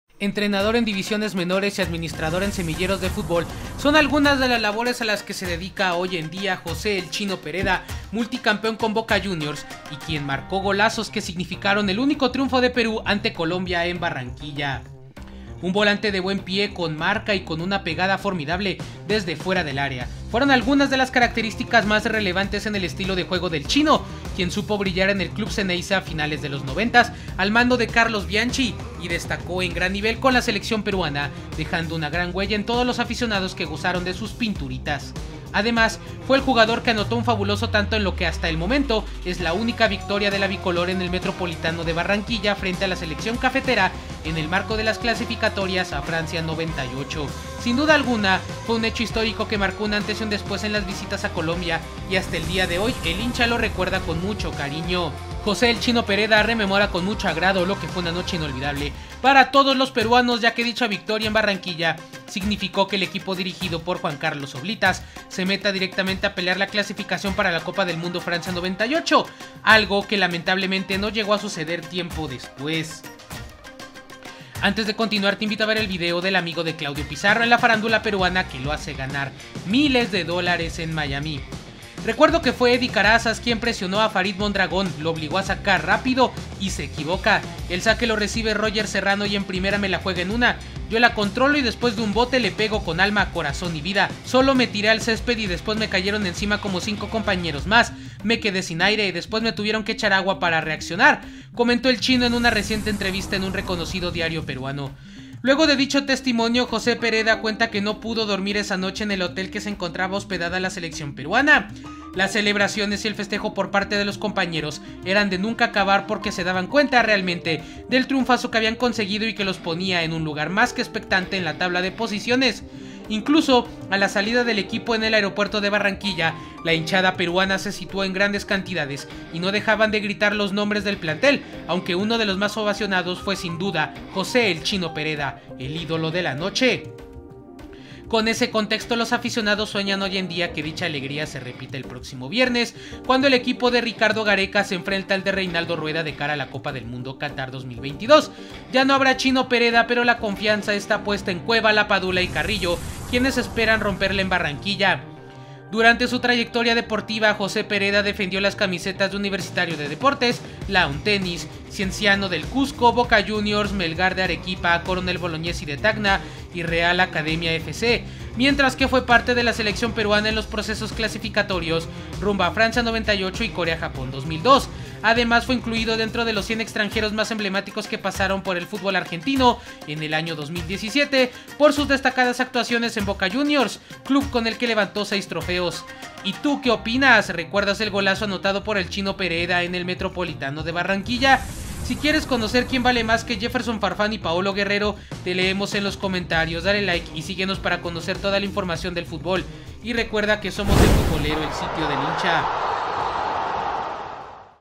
🚨 ¿Qué fue de la vida de JOSÉ PEREDA?🤔
Qué fue de JoséPereda, el autor del gol en el único triunfo de Perú ante Colombia en Barranquilla.